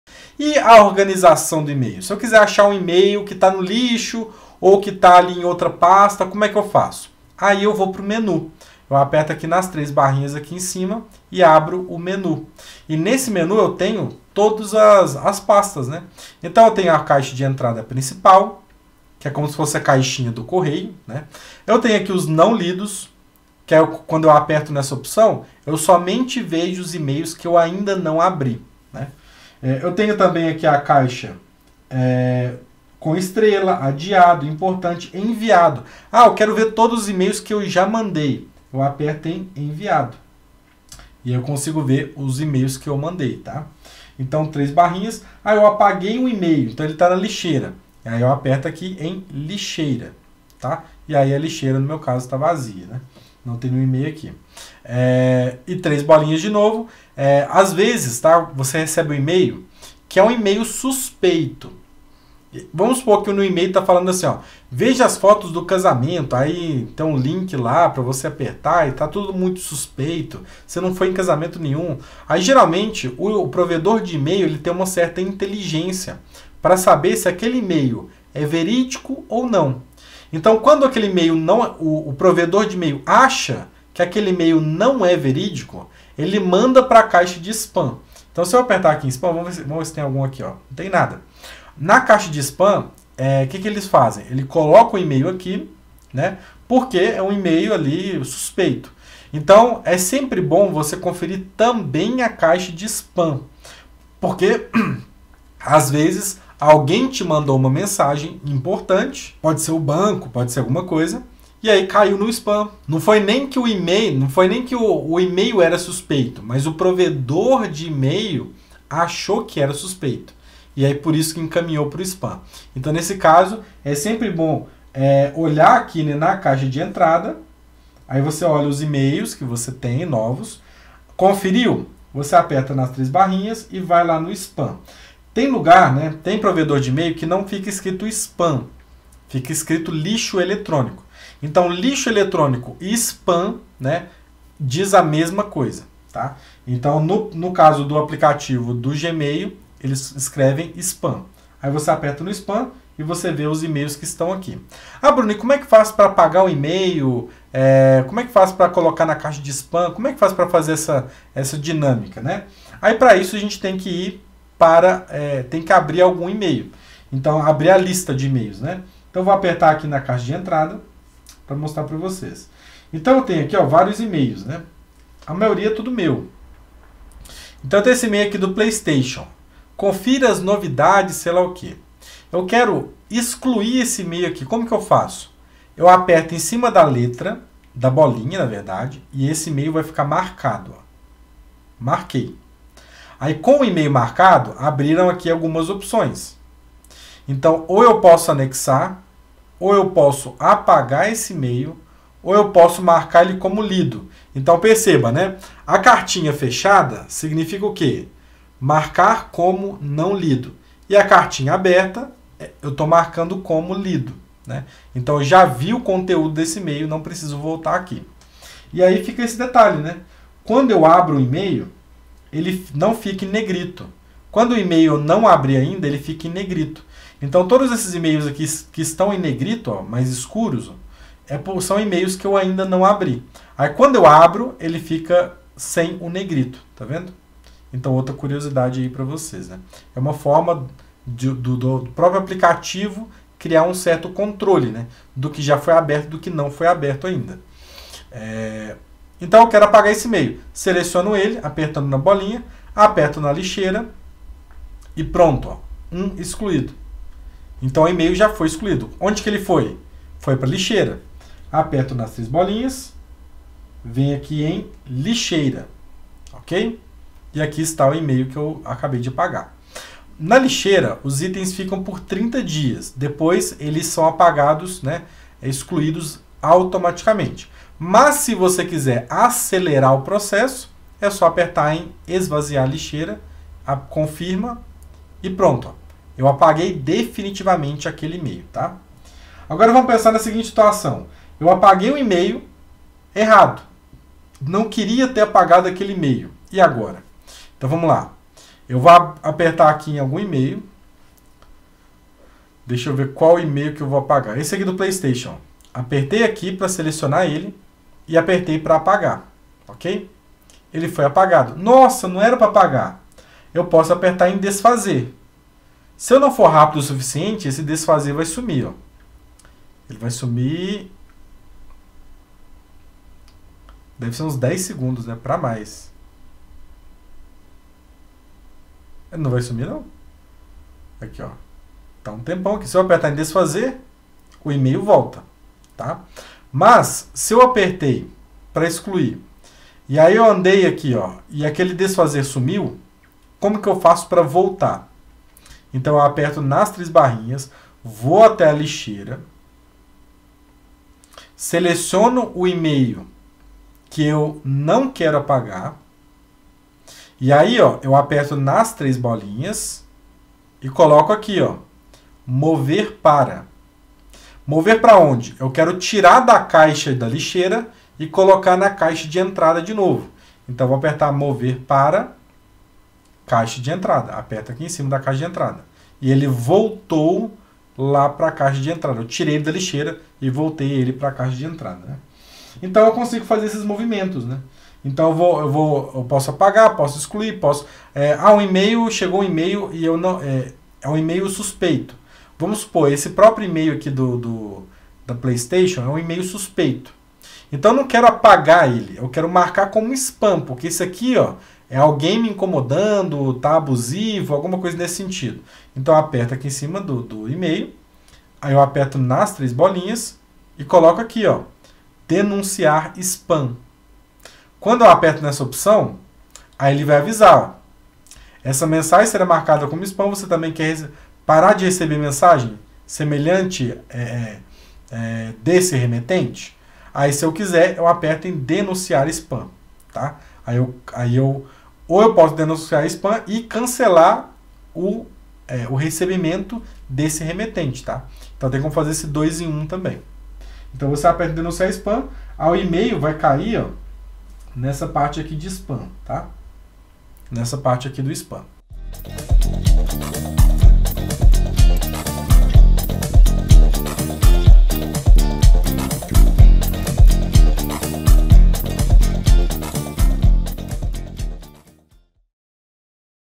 COMO FUNCIONAM AS PASTAS DE E MAILS ENVIADOS, EXCLUÍDOS E SPAM NO GMAIL DO CELULAR?
O aplicativo do Gmail possui algumas pastas padrões para organização. Além da caixa de entrada, que é a pasta principal, também encontramos pastas como a de e-mails enviados, excluídos e spam. Saiba mais sobre como funcionam essas pastas no vídeo de hoje!